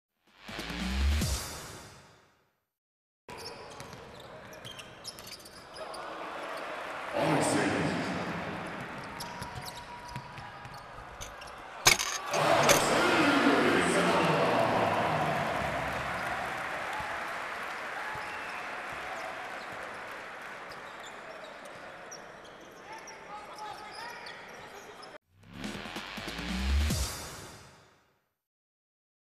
Aleksa Ilić blocks, Aleksa Ilić dunks! (Budućnost VOLI - Petrol Olimpija, 21.1.2018)
This is what Aleksa Ilić does! He jumps, he blocks and he dunks! All in just a couple of seconds!